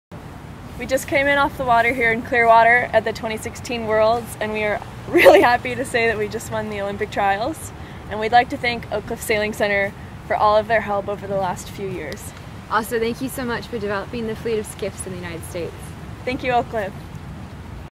Thank You Oakcliff! Paris Henken & Helena Scutt
We have our first 2016 U.S. Olympic Team sailors! Paris and Helena  have won the US SAILING​ Olympic Selection Series in the 49erFX (women's skiff), and will join Team USA​ in Rio 2016​!

They could not have done it without Oakcliff Sailing (Oyster Bay, N.Y.), an official US Sailing Team Sperry Training Center.